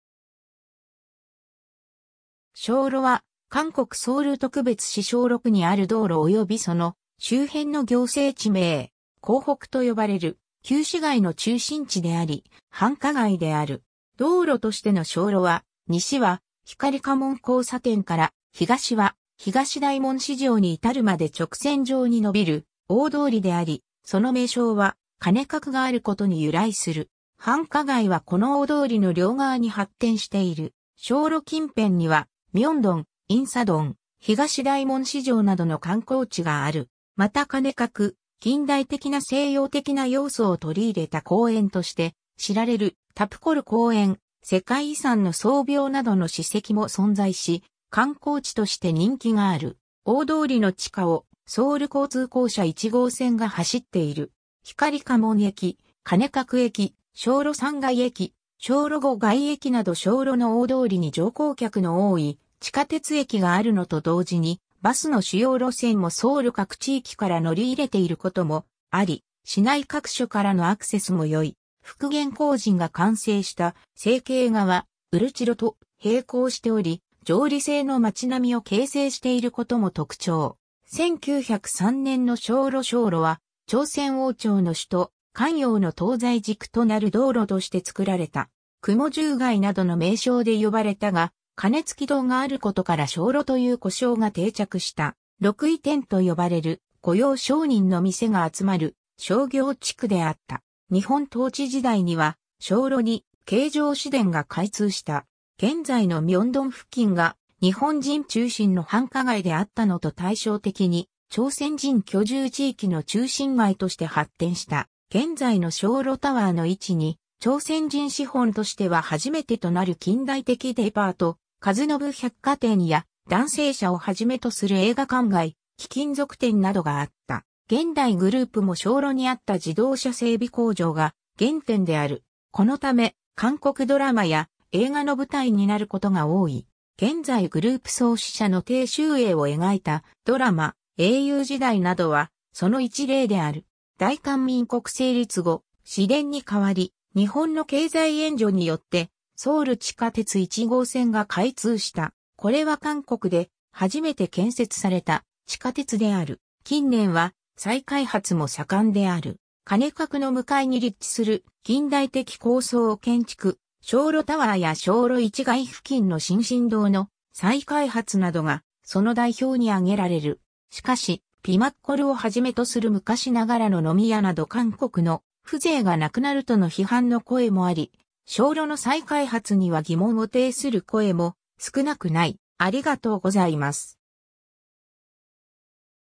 鍾路
鍾路 （地名：チョンノ 道路名：チョンロ）は韓国ソウル特別市鍾路区にある道路およびその周辺の行政地名。
江北（漢江の北側）と呼ばれる旧市街の中心地であり、繁華街である。
 道路としての鍾路は、西は光化門交差点から東は東大門市場に至るまで直線状に伸びる大通りであり、その名称は鐘閣（普信閣）があることに由来する。
繁華街はこの大通りの両側に発展している。
鍾路近辺には明洞、仁寺洞、東大門市場などの観光地がある。
また鐘閣、近代的な西洋的な要素を取り入れた公園として知られるタプコル（パゴダ）公園、世界遺産の宗廟などの史跡も存在し、観光地として人気がある。
 大通りの地下をソウル交通公社1号線が走っている。
光化門駅、鐘閣駅、鍾路3街駅、鍾路5街駅など鍾路の大通りに乗降客の多い地下鉄駅があるのと同時にバスの主要路線もソウル各地域から乗り入れていることもあり、市内各所からのアクセスもよい。
復元工事が完成した清渓川、乙支路と平行しており、条里制の街並みを形成していることも特徴。
 1903年の鍾路 鍾路は、朝鮮王朝の首都・漢陽の東西軸となる道路として作られた。
雲従街などの名称で呼ばれたが、鐘撞き堂（鐘閣，即ち普信閣）があることから鍾路（鐘路）という呼称が定着した。
六矣廛と呼ばれる御用商人の店が集まる商業地区であった。
 日本統治時代には、鍾路に京城市電が開通した。
現在の明洞付近が日本人中心の繁華街であったのと対照的に朝鮮人居住地域の中心街として発展した。
現在の鍾路タワー（ミレニアムタワー）の位置に朝鮮人資本としては初めてとなる近代的デパート・和信百貨店（ファシン-）や、「団成社」をはじめとする映画館街、貴金属店などがあった。
現代グループも鍾路にあった自動車整備工場が原点である。
このため、韓国ドラマや映画の舞台になることが多い。
現在グループ創始者の鄭周永を描いたドラマ「英雄時代」などはその一例である。
 大韓民国成立後、市電に代わり、日本の経済援助によってソウル地下鉄1号線が開通した。
これは韓国で初めて建設された地下鉄である。
 近年は再開発もさかんである。
鐘閣の向かいに立地する近代的高層建築、鍾路タワー（もともと経済危機前にサムスングループが総合ショッピングセンター建設を目的に工事が始まったが、1997年末におきた経済危機で頓挫、一時期、韓国国税庁が建物を間借りしていたこともある）や鍾路1街付近の清進洞の再開発などがその代表に挙げられる。
 しかし、ピマッコルをはじめとする昔ながらの飲み屋など韓国の風情がなくなるとの批判の声もあり、鍾路の再開発には疑問を呈する声も少なくない。